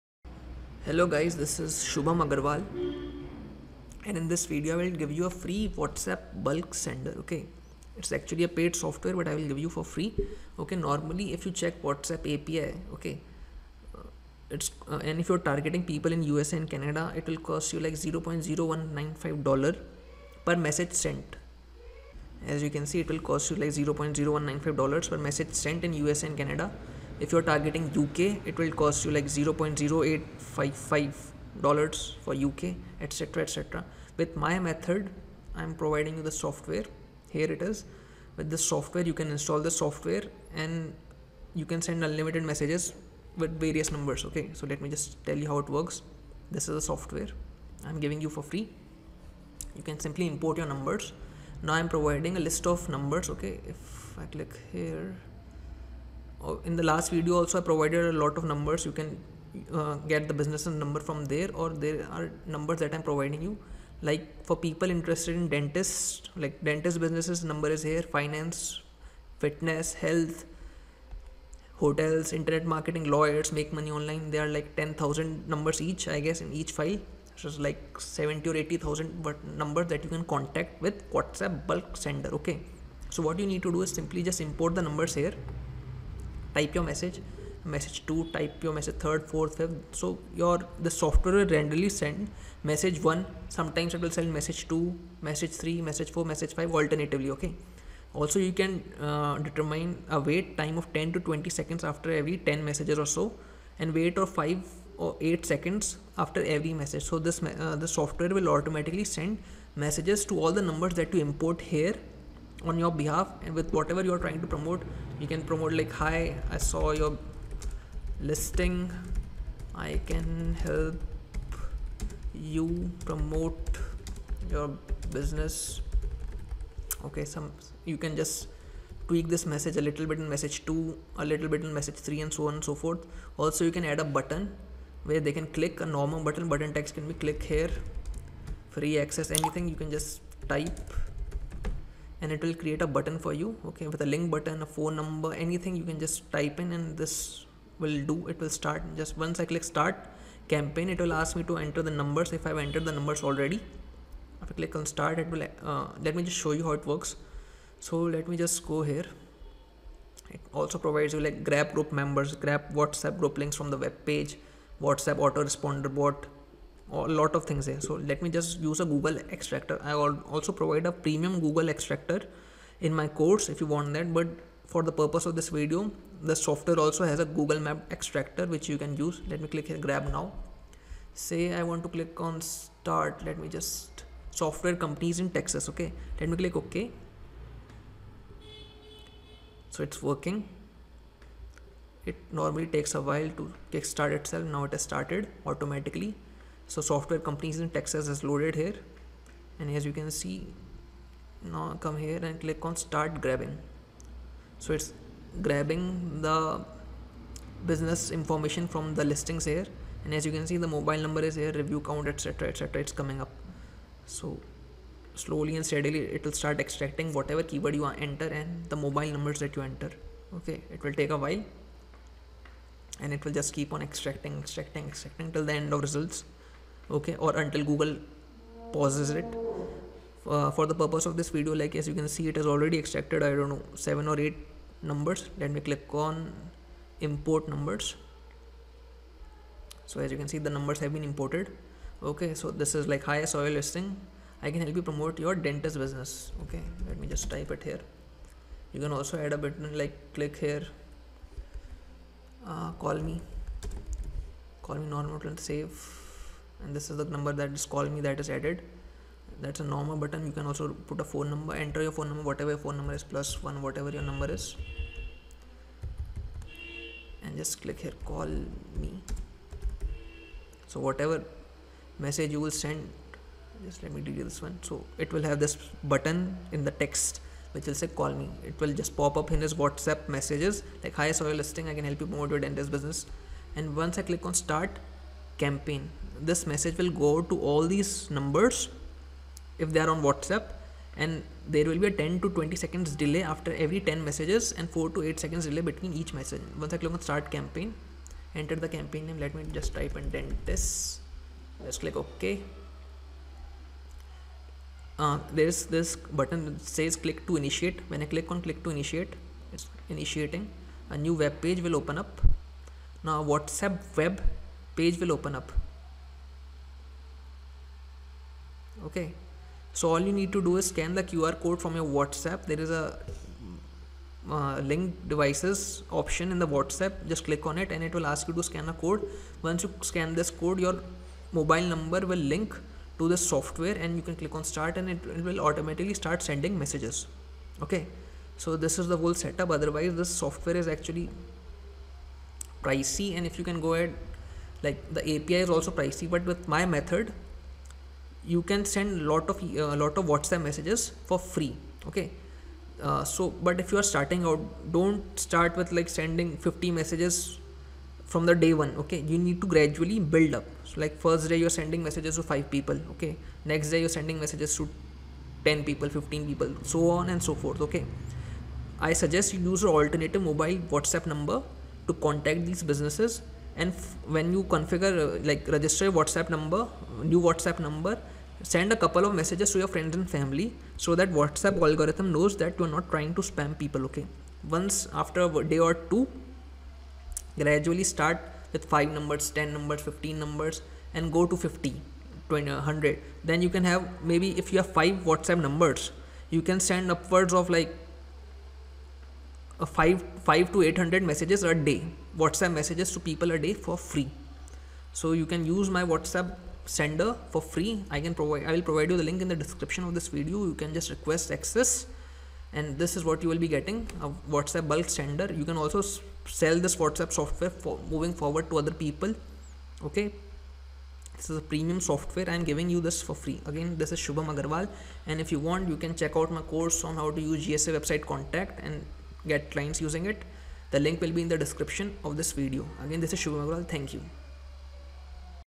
Free Whatsapp Bulk Message Sender
In this video, I'll give you free Whatsapp Bulk Message Sender.
This is a premium software, but for you all I am giving it for free.

Plus you'll get roughly 80000 mobile number list of different niches.

If you're interested in GSA Website Contact training, you can check my course.

See Ya
Agrawal Bros